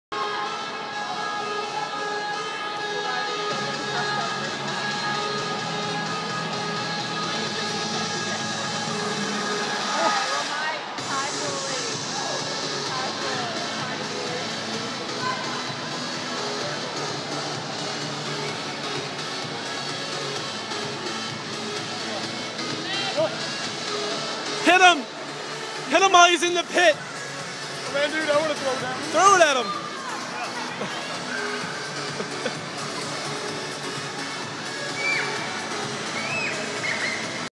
Rebounderz 2013
This is an indoor trampoline Arena in Edison, New Jersey. This is me doing a flip into their foam pit!